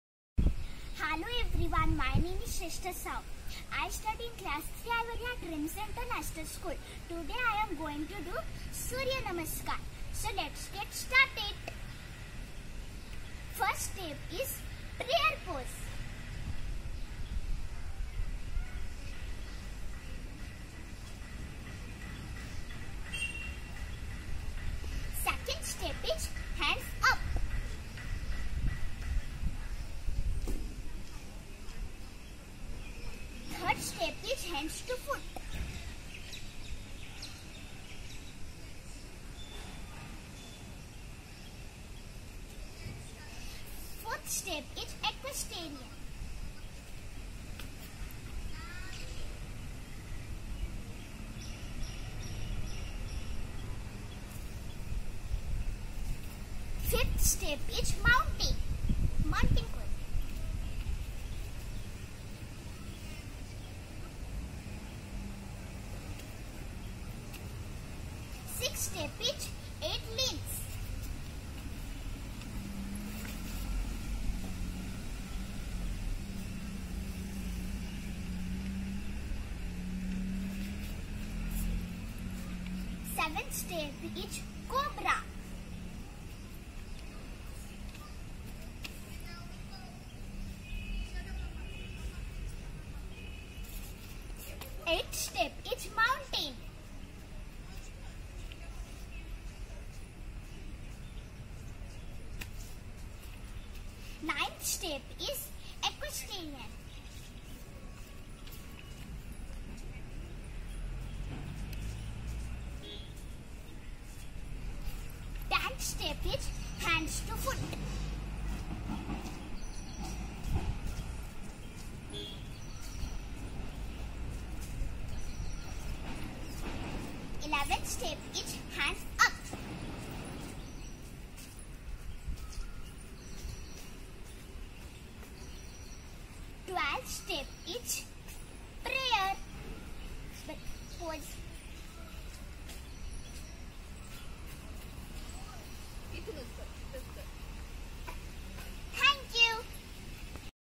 Surya Namaskar for kids by Shrestha Sahu🧘‍♀️🧘 .. /International yoga day Special . //Surya namaskar😊
Surya namaskar easy steps & Best and easy yoga for kids and children , in daily routine "surya namskar"  or Sun Salutation. In this video I do Surya Namaskar . International yoga day Special .